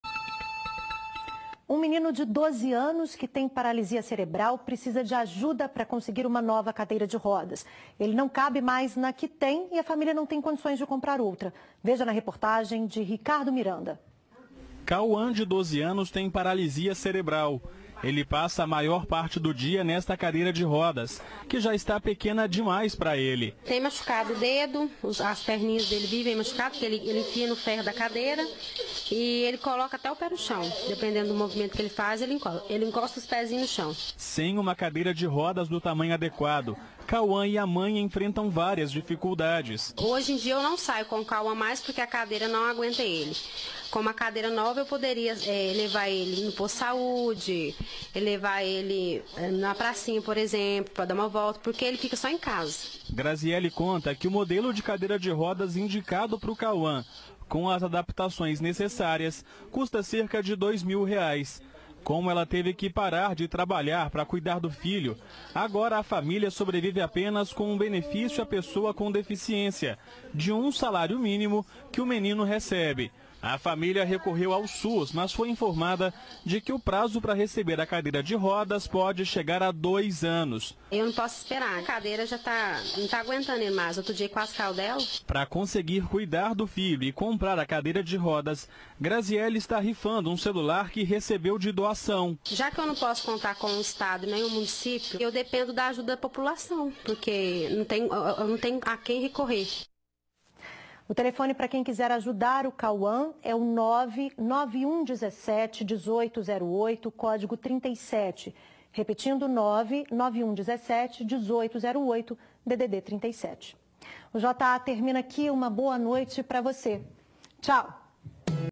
Menino com paralisia cerebral precisa de cadeira de rodas
Um menino de doze anos, que tem paralisia cerebral, precisa de ajuda para conseguir uma nova cadeira de rodas.